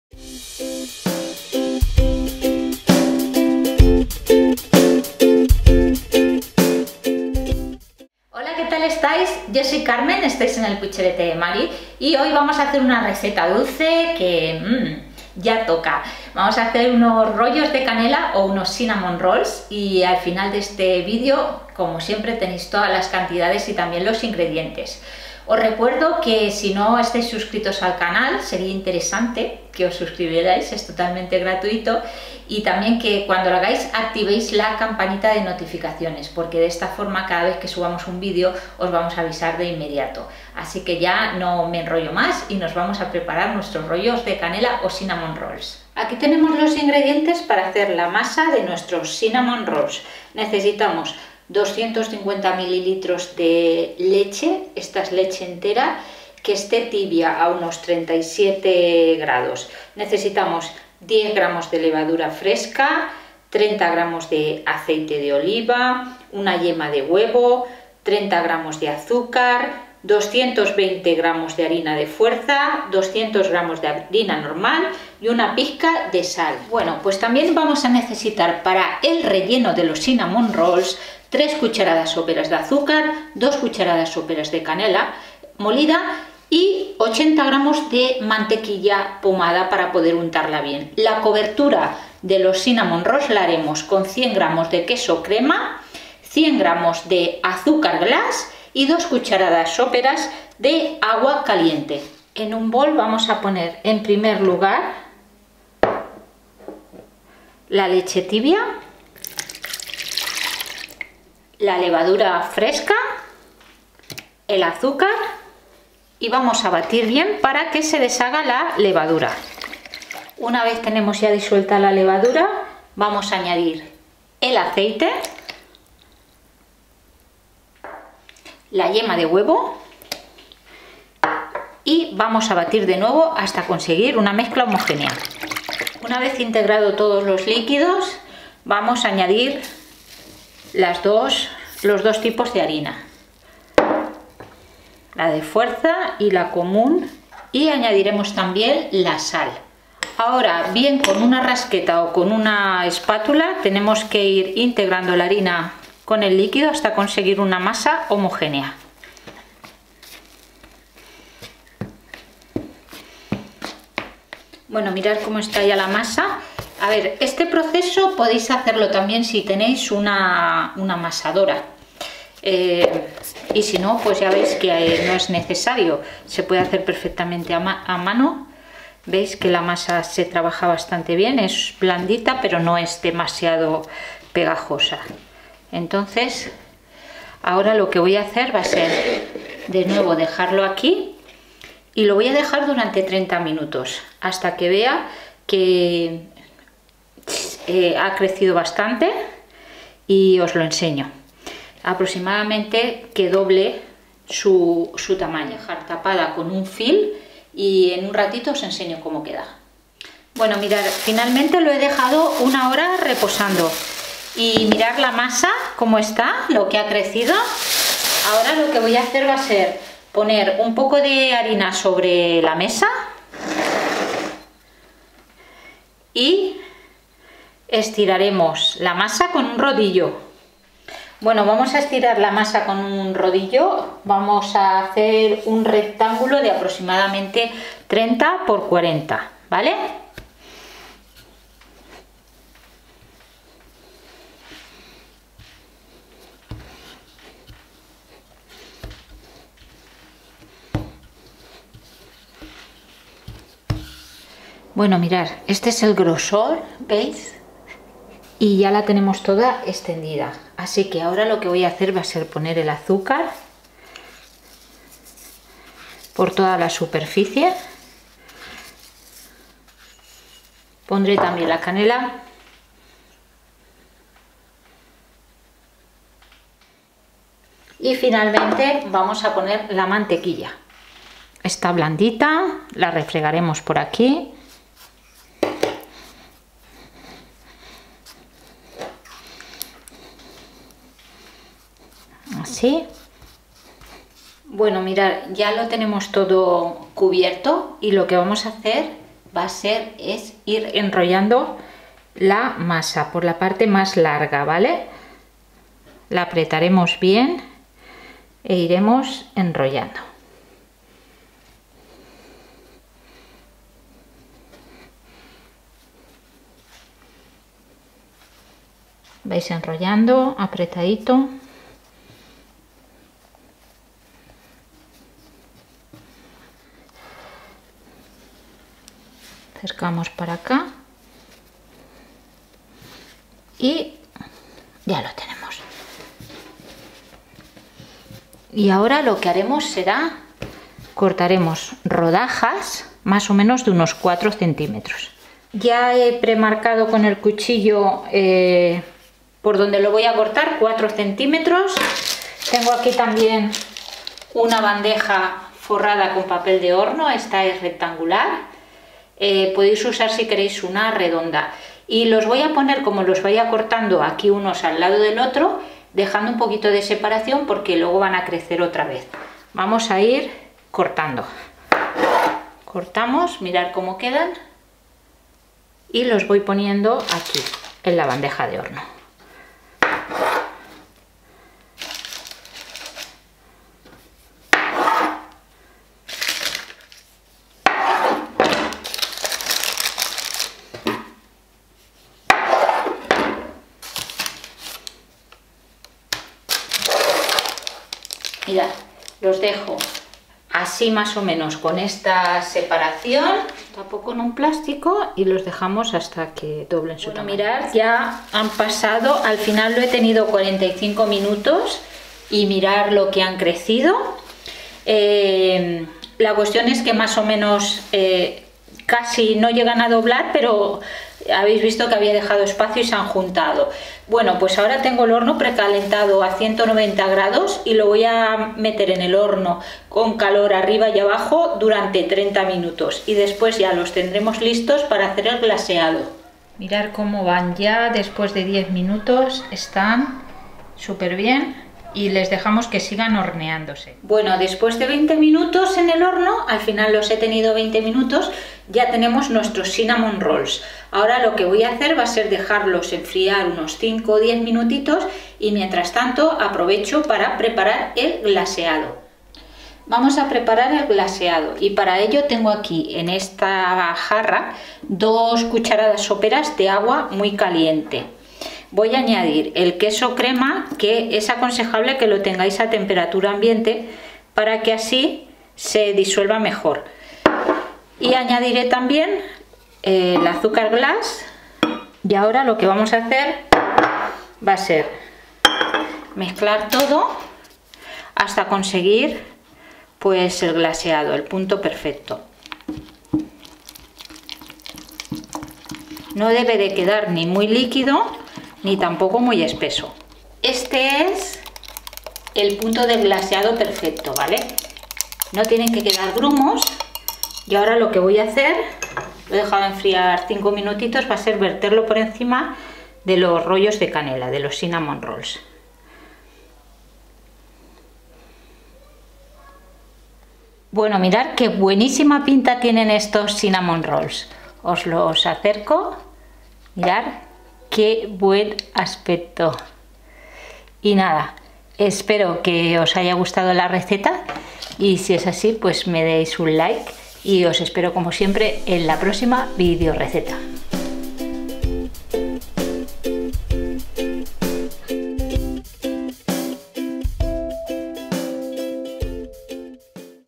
CINNAMON ROLLS - Los famosos ROLLITOS DE CANELA | Episodio 222
Os presentamos una nueva video-receta: Cynamon Rolls (Rollos de canela) un dulce muy famoso al que es difícil resistirse porque son adictivos. Su textura es tierna y su relleno una explosión de sabor. Cuando muerdas uno de mis cynamon rolls vas a salivar porque el glaseado es una pasada. Espero que os guste.

INGREDIENTES
Para la masa:
- 250 ml de leche

- 10 g de levadura fresca
- 30 g de aceite

- 1 yema de huevo

- 30 g de azúcar

- 220 g de harina de fuerza
- 200 g de harina todo uso

- 1 pizca de sal

Para el relleno:
- 3 cucharadas soperas de azúcar
- 2 cucharadas soperas de canela
- 80 g de mantequilla pomada

Para el glaseado:

- 100 g de queso crema
- 100 g de azúcar glass
- 2 cucharadas de agua caliente.

Precalentar y hornear a 190ºC, calor arriba y abajo,
durante 20 min.